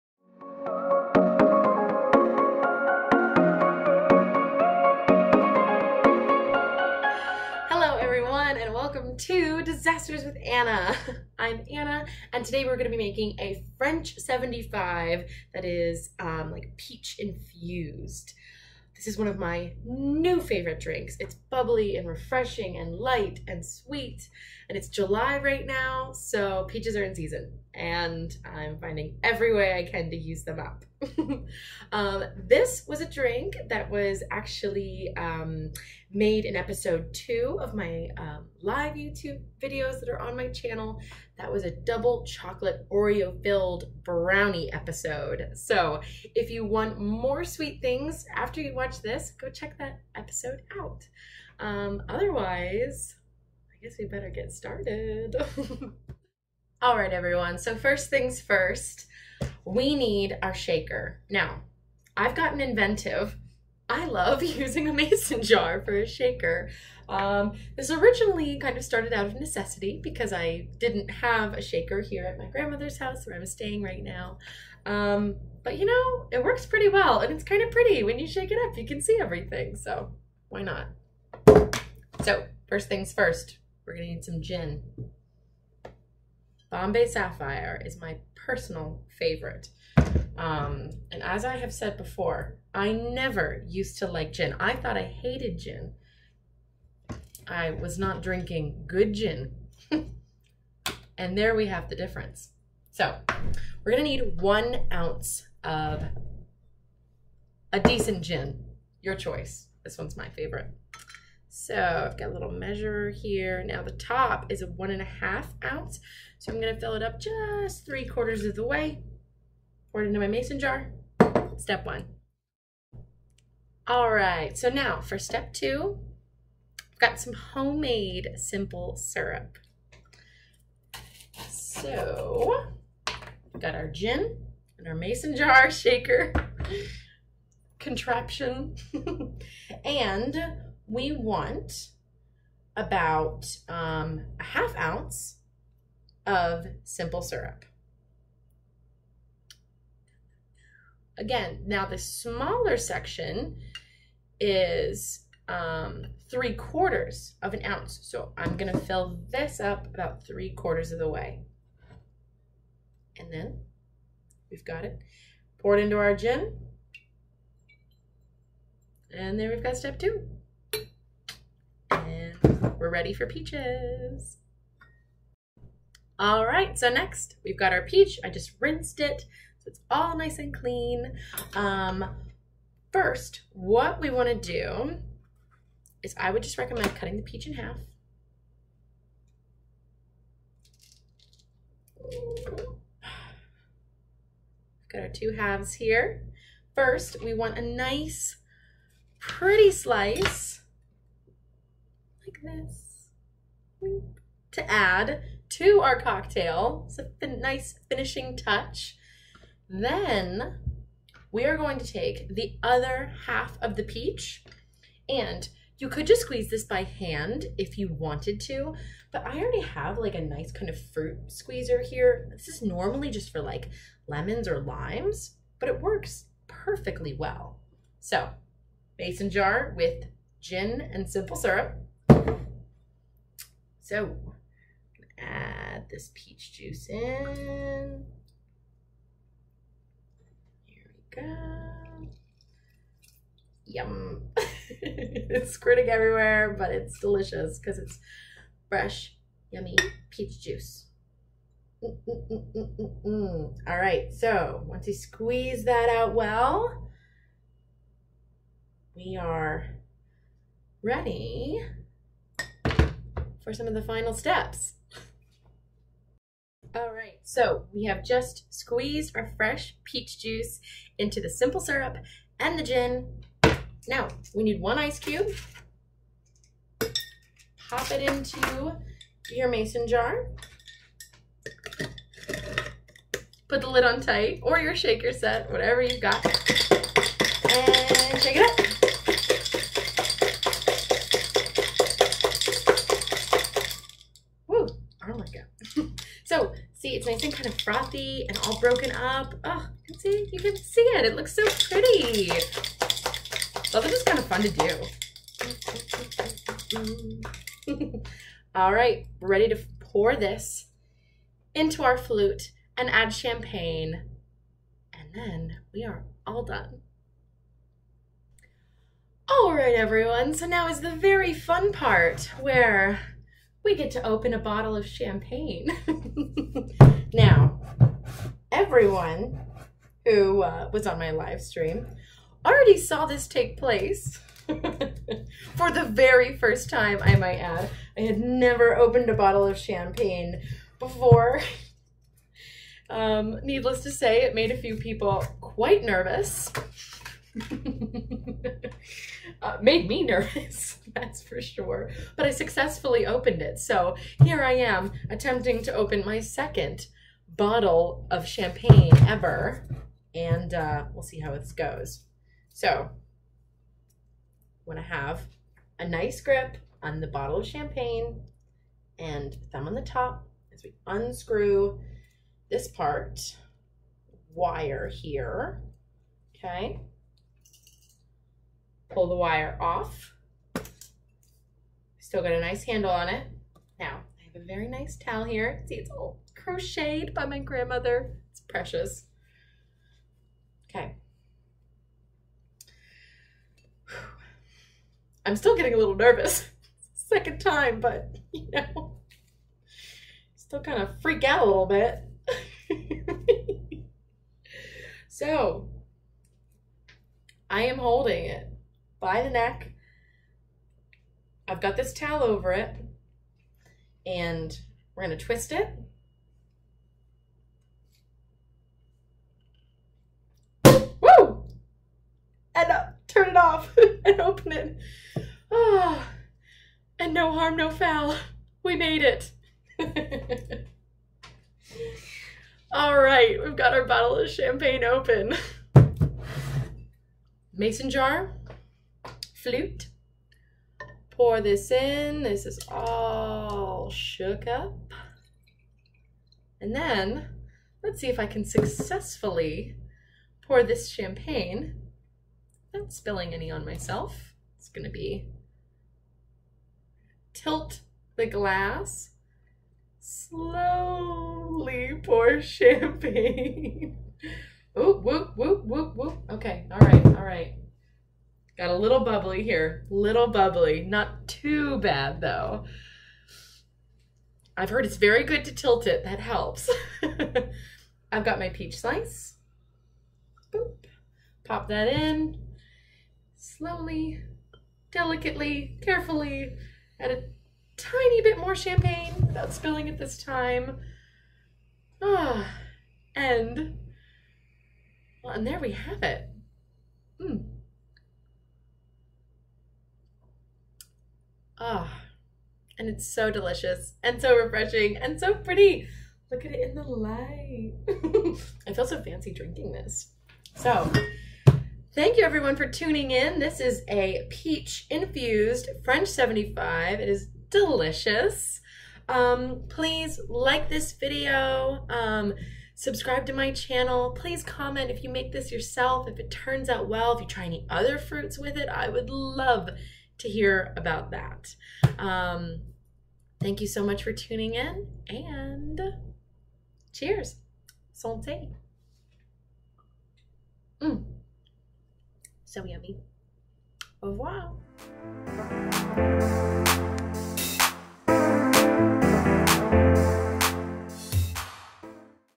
Peach Infused French 75 Cocktail
This is a light and bubbly drink that is the perfect combination of herbaceous and sweet!

This would be a great drink to try with different fruits of your choice too, I might be trying mine with raspberries next.

This is my first recorded and edited video for my channel and I’m so excited to share this with you.

Ingredient Details
- 1 fresh peach
- 1 oz gin
- bottle of champagne (if you just want to make a glass or two a mini champagne bottle would work too! )